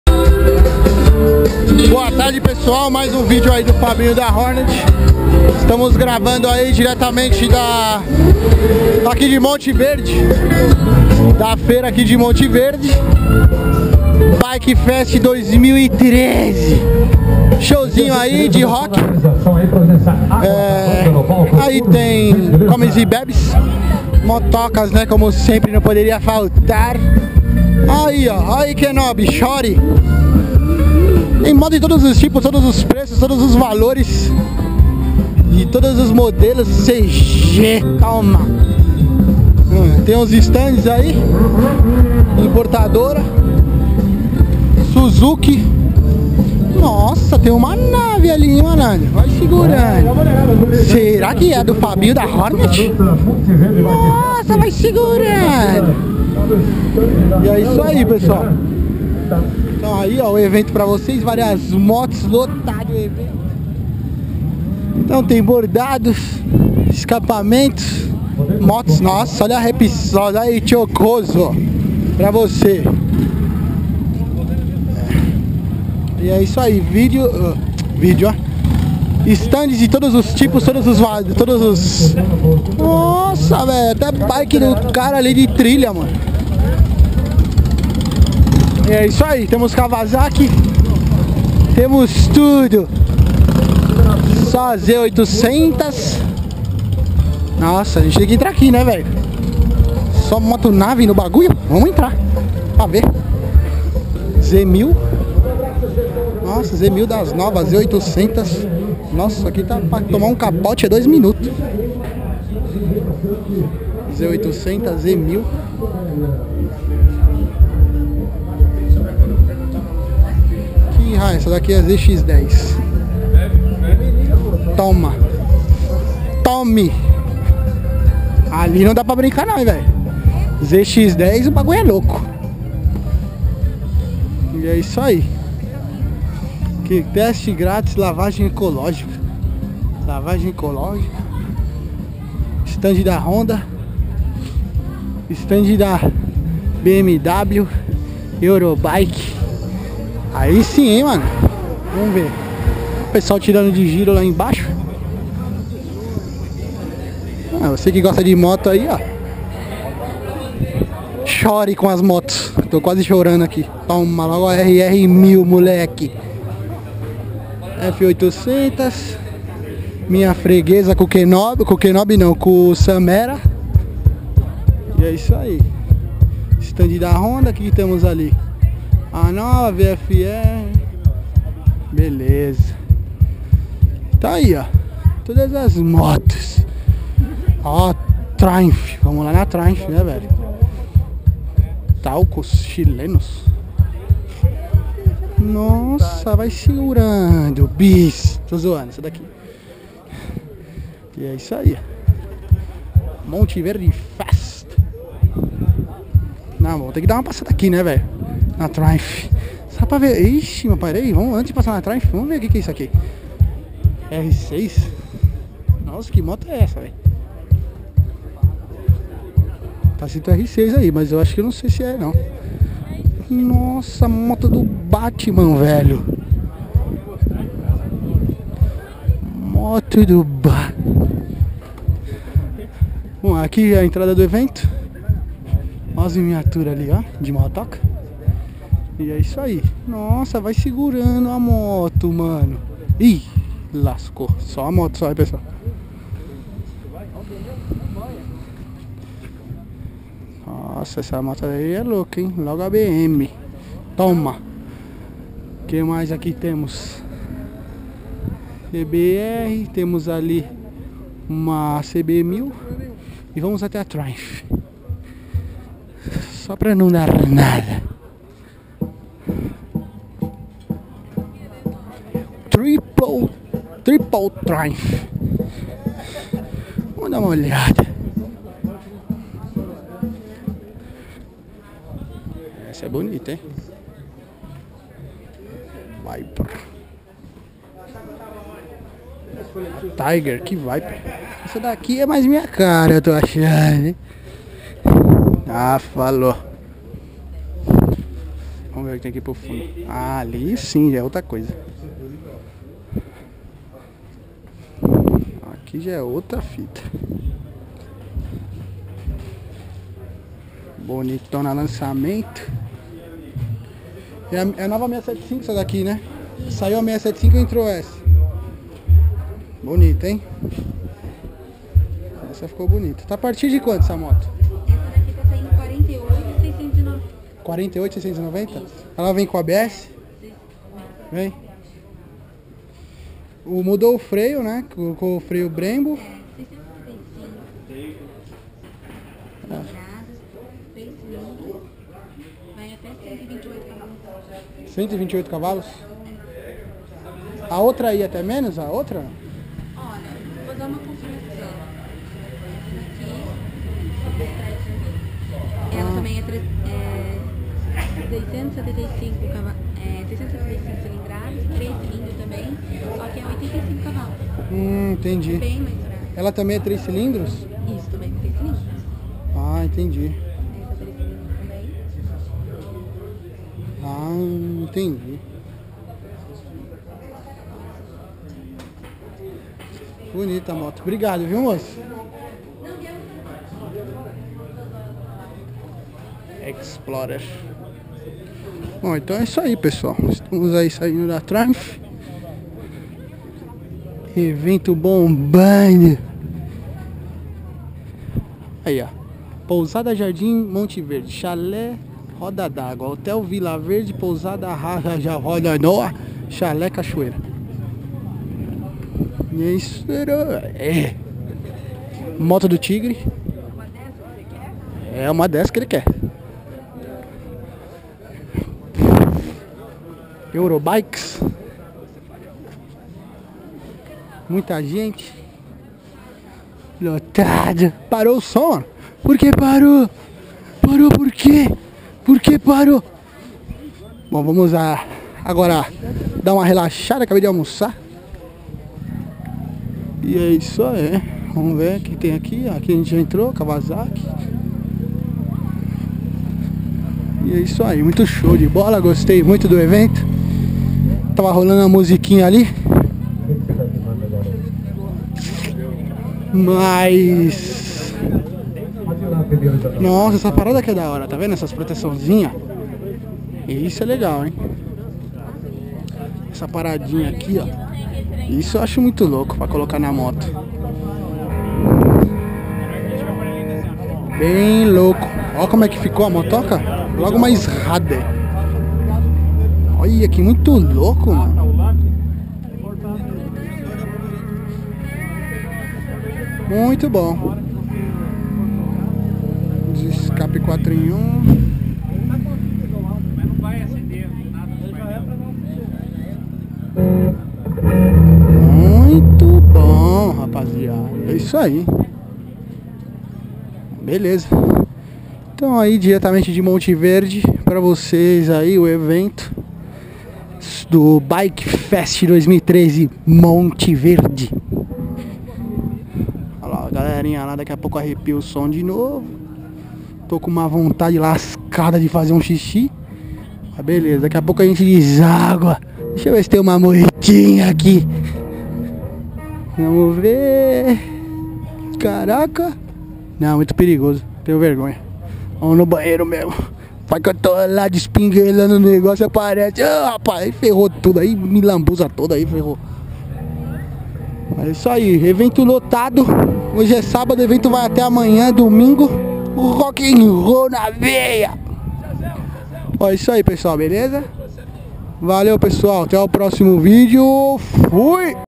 FABINHO DA HORNET - MOSTRANDO O EVENTO BIKE FAST 2013 - LOJAS DA KAWASAKI, TRIUMPH E BWM
Mostrando o Evento de Ponta a Ponta!